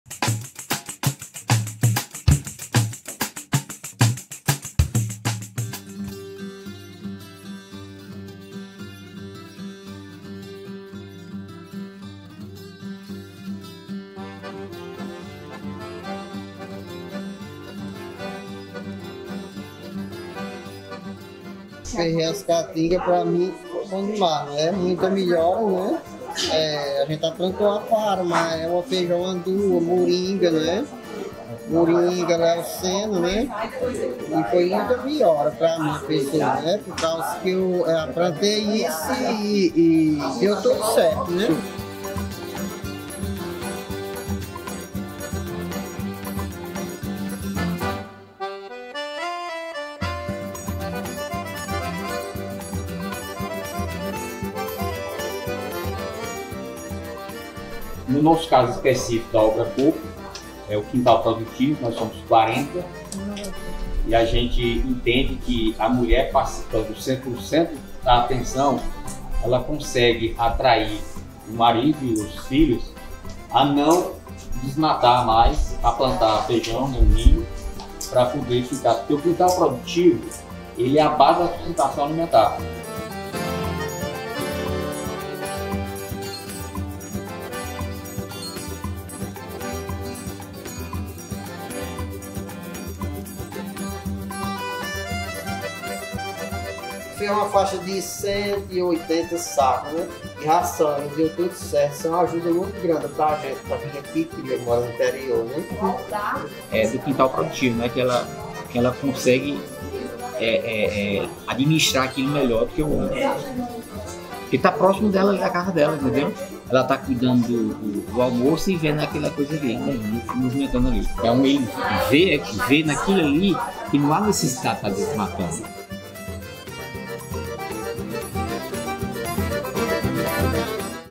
Obra Kolping
A Obra Kolping Piauí atua em Betânia do Piauí, com 90 famílias beneficiárias. Os agricultores têm como foco a criação de pequenos animais e a lavoura, com cultivo de feijão, milho, canteiros de hortaliças e plantas medicinais